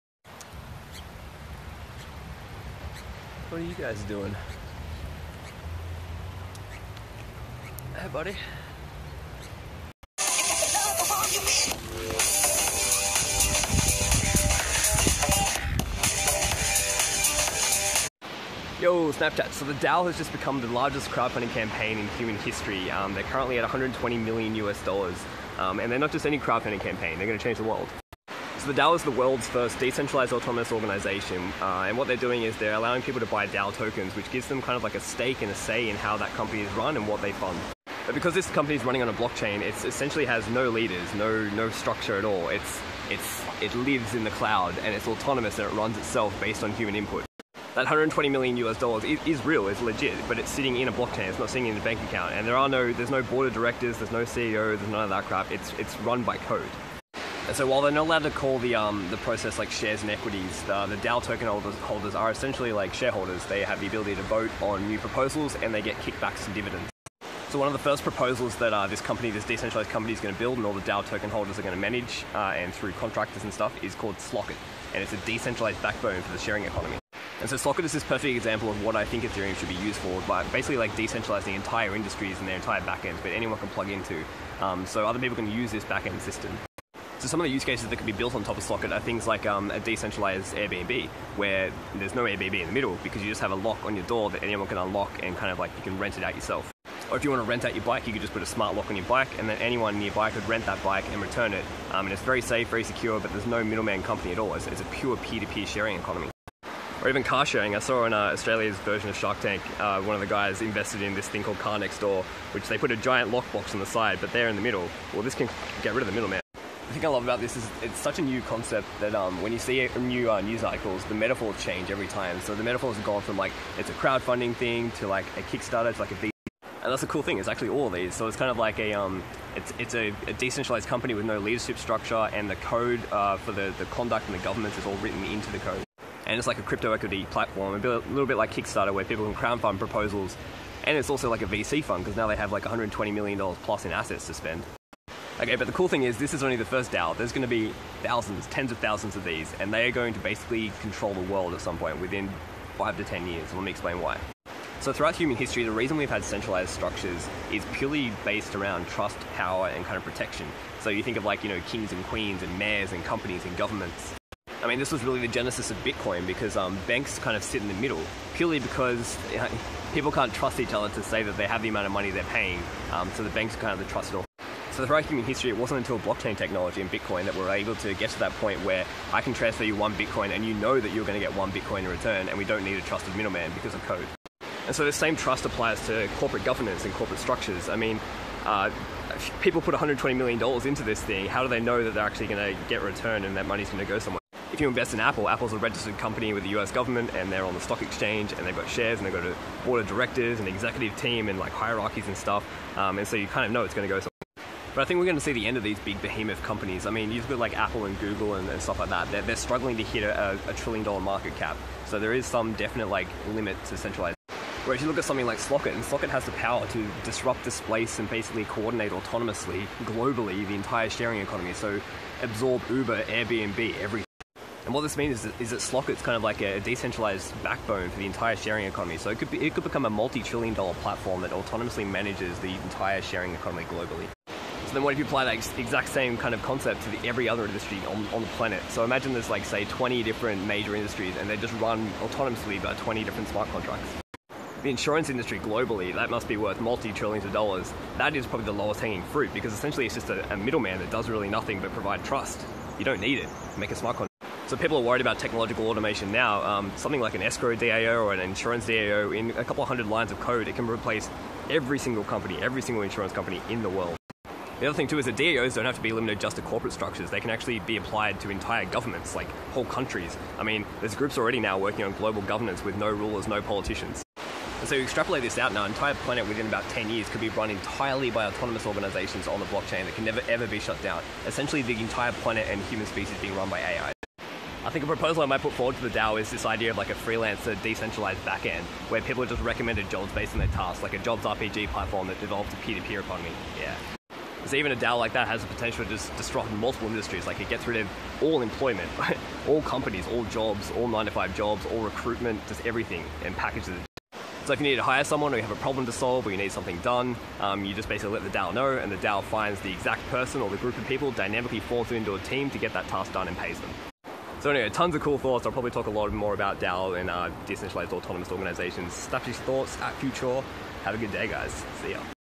The DAO
USD$120M and counting. The DAO is the largest crowdfunding campaign in history and is set to decentralize the world